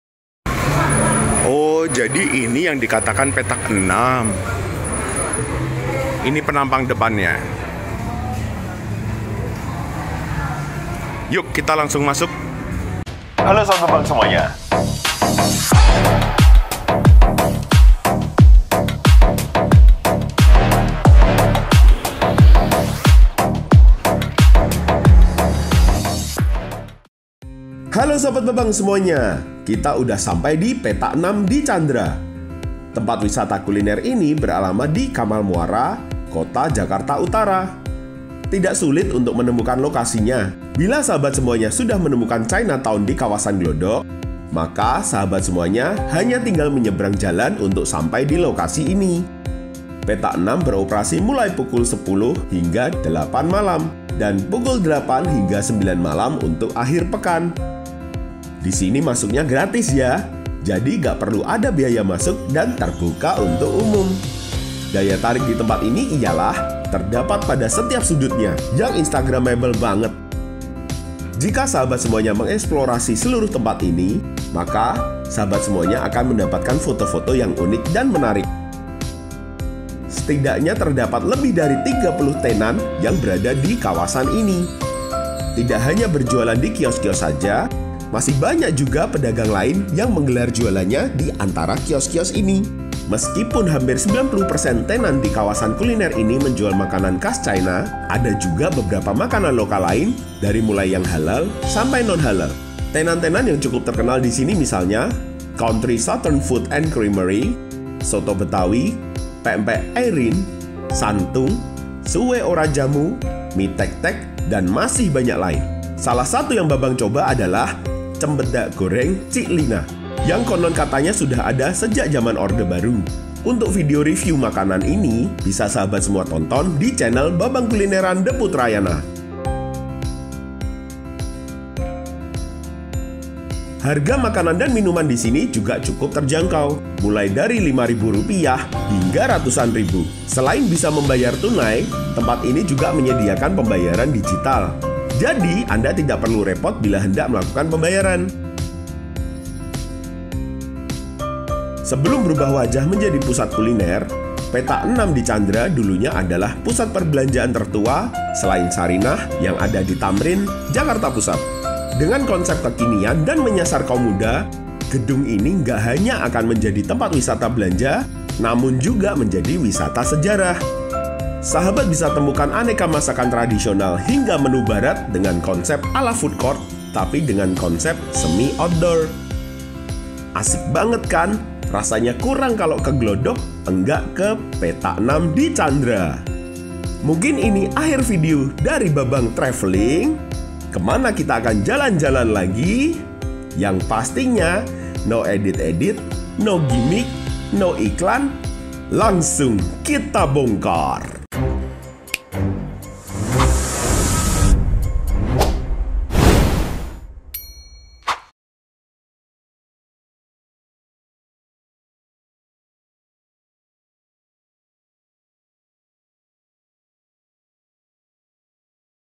Gedung Pusat Belanja Tua di Jakarta menjadi destinasi wisata kuliner Glodok Pancoran
Petak Enam di Chandra merupakan salah satu pusat kuliner baru di kawasan Glodok, Jakarta Barat.

Petak Enam menawarkan lebih dari 30 tenant yang menawarkan aneka sajian. Ada kuliner khas China, peranakan, Melayu, Barat, Italia, hingga tradisional Indonesia.

Pastikan datang tidak kesorean !

Banyak tenant yang bisa menjual makanannya dengan cepat, salah satunya adalah Cempedak Goreng Cik Lina.

Biasanya kedai tersebut sudah tutup sejak pukul 15.00 – 16.00 WIB karena stok cempedak yang terbatas.

Maka dari itu, usahakan datang lebih awal. Petak Enam buka Senin – Jumat pukul 10.00 – 20.00 WIB dan Sabtu – Minggu pukul 08.00 – 21.00 WIB.

Namun, biasanya banyak tenant yang sudah tutup sejak pukul 18.00 WIB.

terletak di Jalan Pancoran No.43, RW.6, Glodok, Kecamatan Taman Sari, Jakarta Barat.
buka Senin – Jumat pukul 10.00 – 20.00 WIB
Sabtu – Minggu pukul 08.00 – 21.00 WIB.